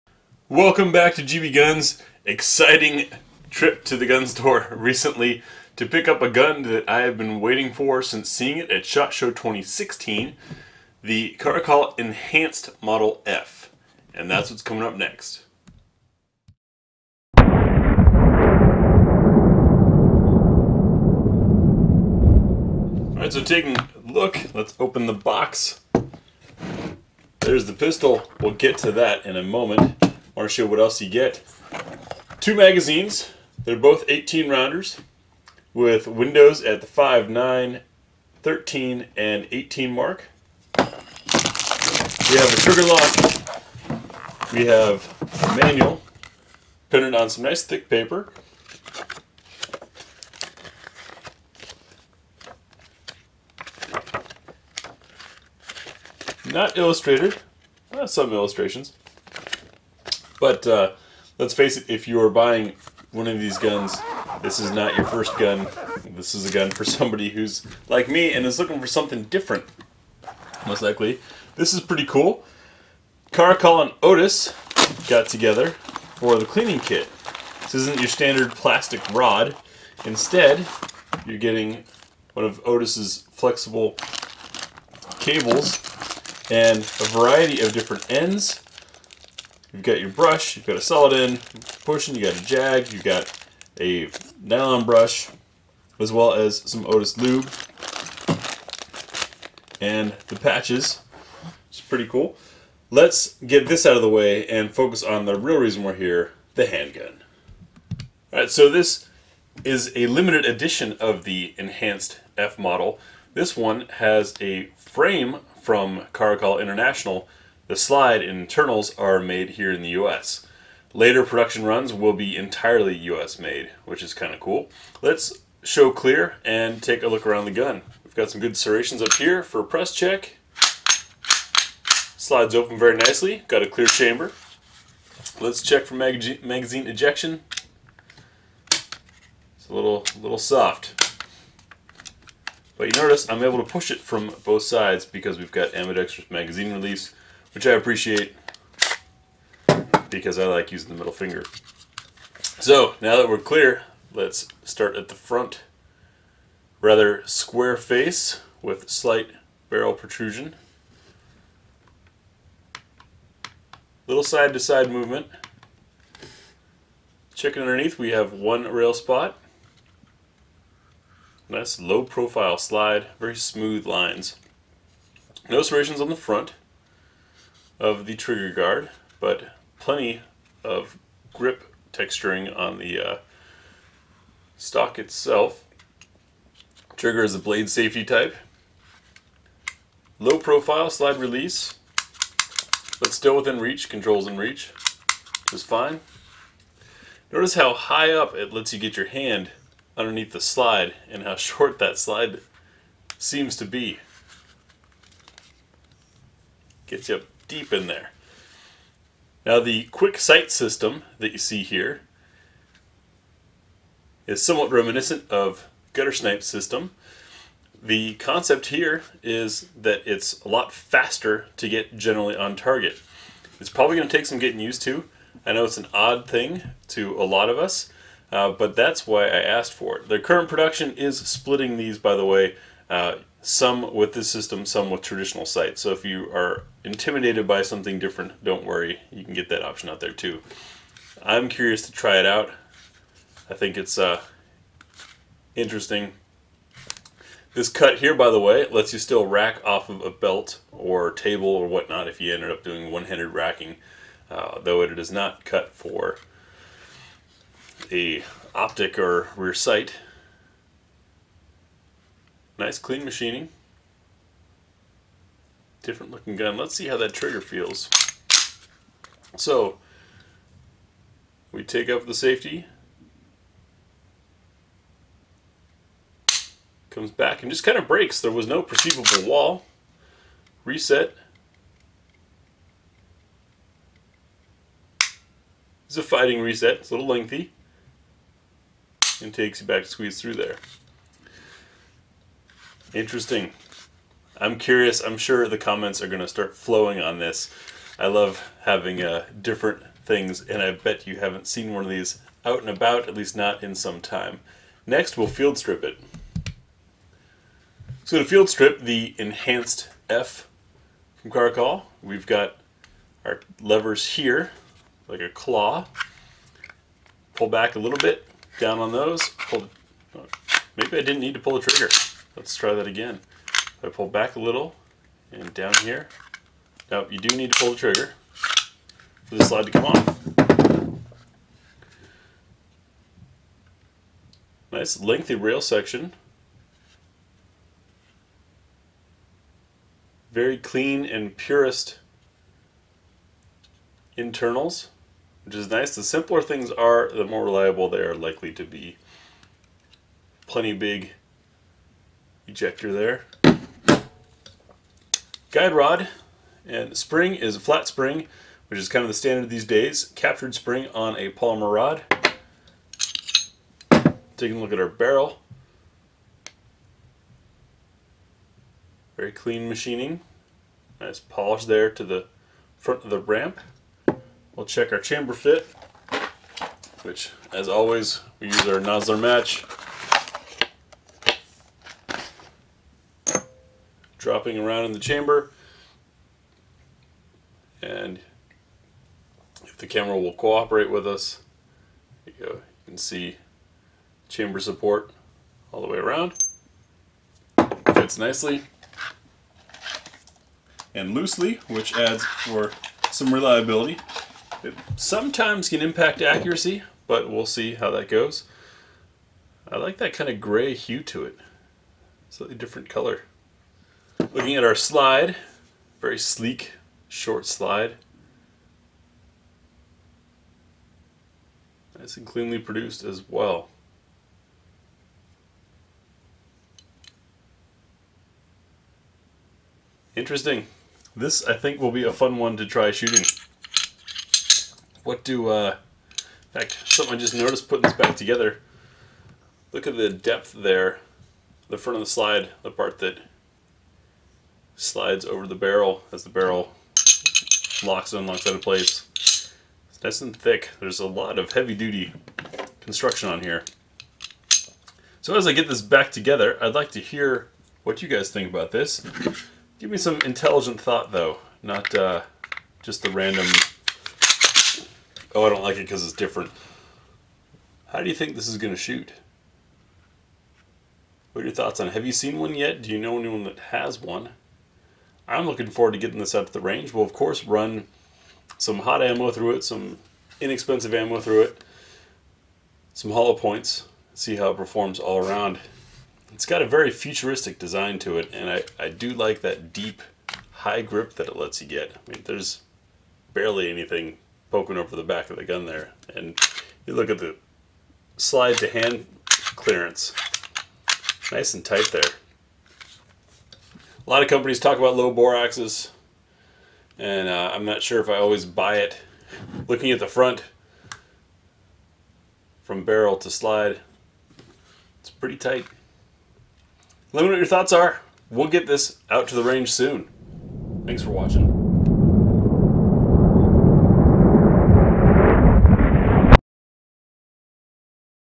Caracal Limited Edition Enhanced F Unbox and Field Strip
After a lengthy wait the Caracal Enhanced F is available.  We take a look at a Limited Edition variant.